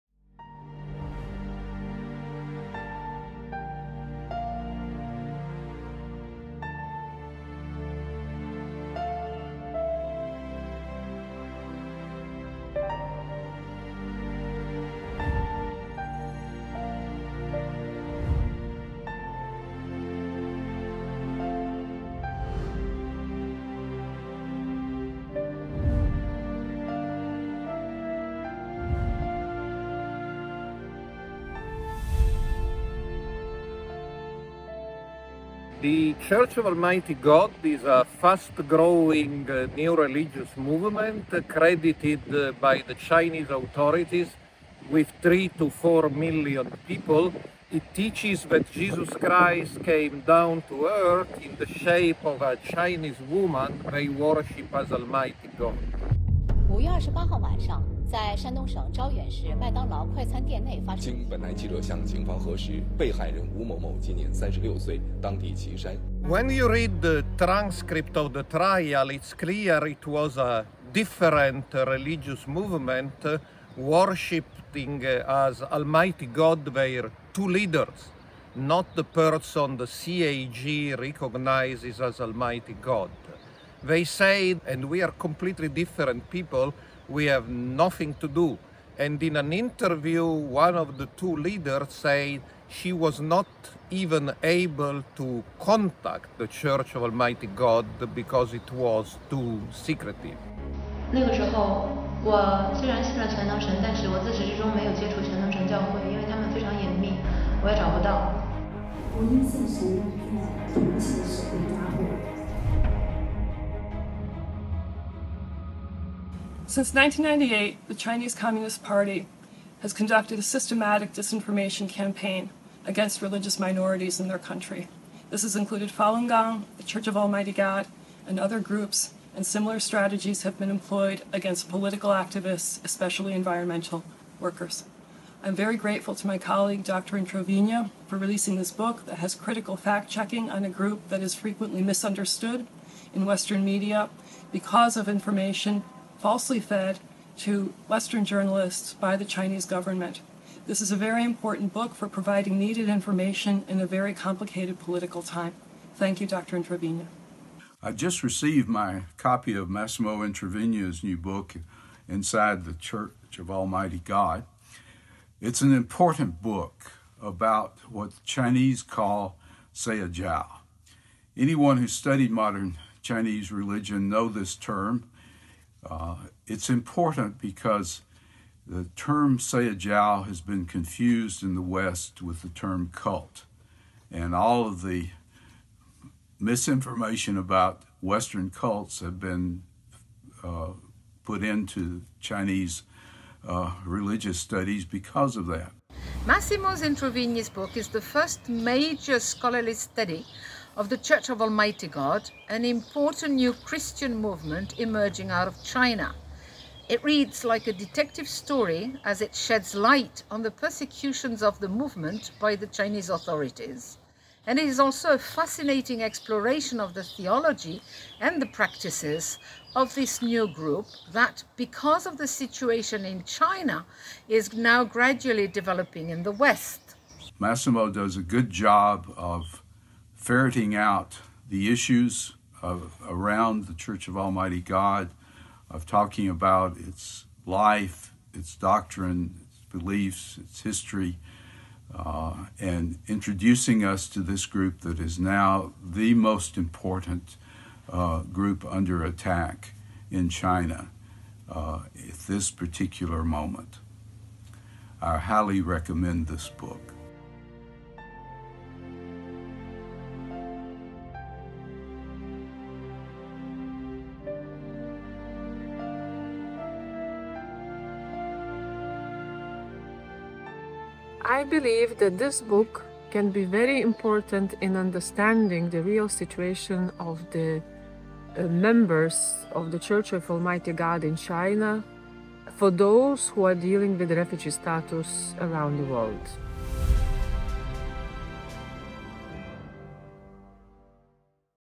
Massimo Introvigne, "Inside The Church of Almighty God": Book Trailer
Bitter Winter has already introduced to its readers the new book by its editor-in-chief Massimo Introvigne, Inside The Church of Almighty God: The Most Persecuted Religious Movement in China, published in 2020 by Oxford University Press.

You're welcome to circulate our video. Please credit Bitter Winter as the source and link to Bitter Winter site.